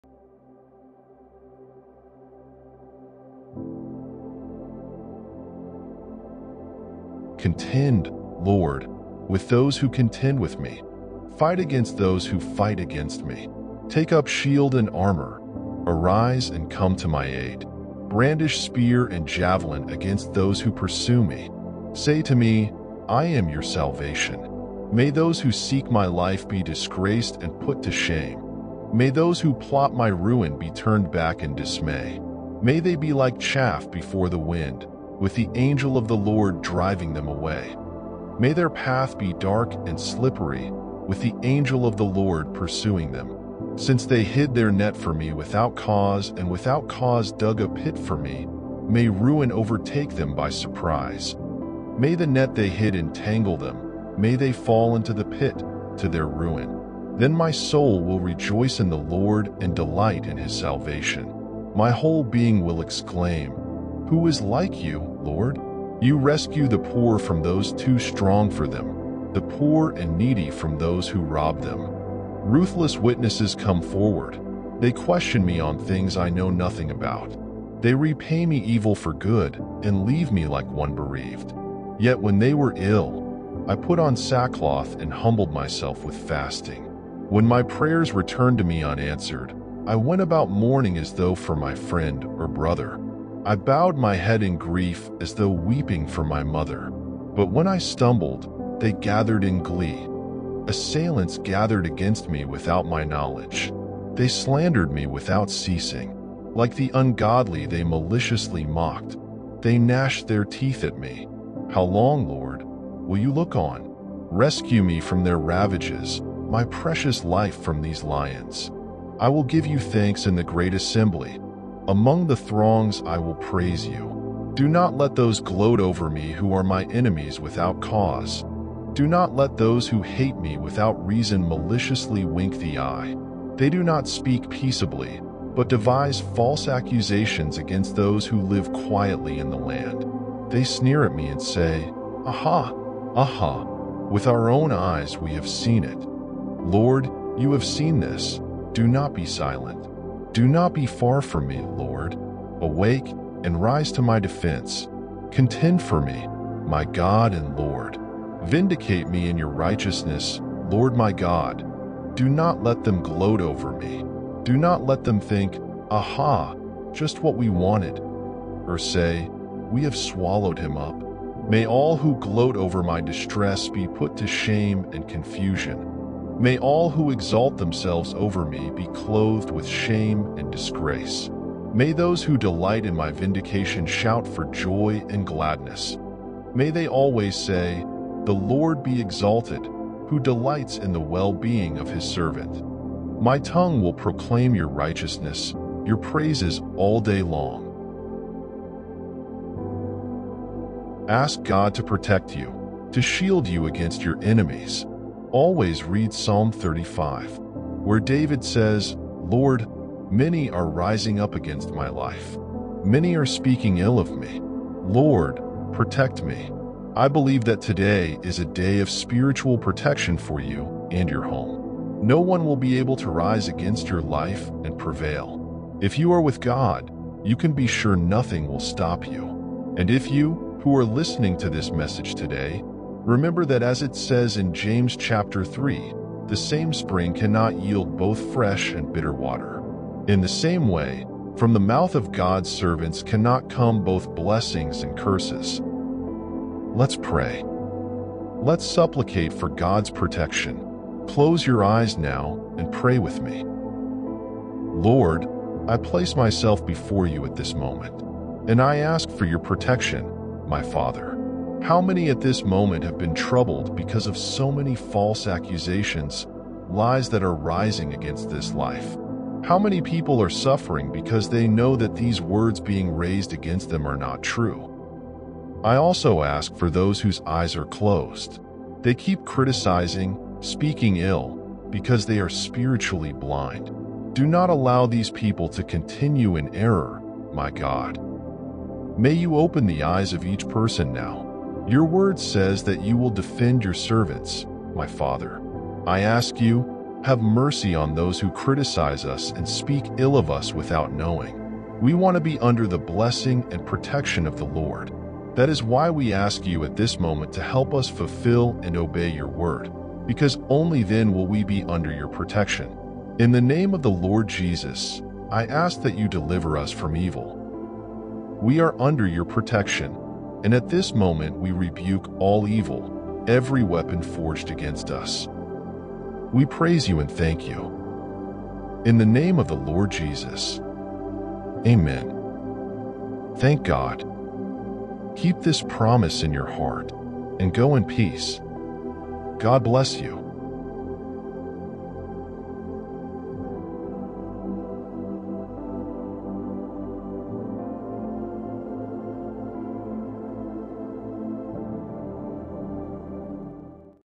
PSALM 35 PRAYER - FOR PROTECTION AGAINST ENEMIES
ASK GOD TO PROTECT YOU, TO SHIELD YOU AGAINST YOUR ENEMIES.
ALWAYS READ PSALM 35.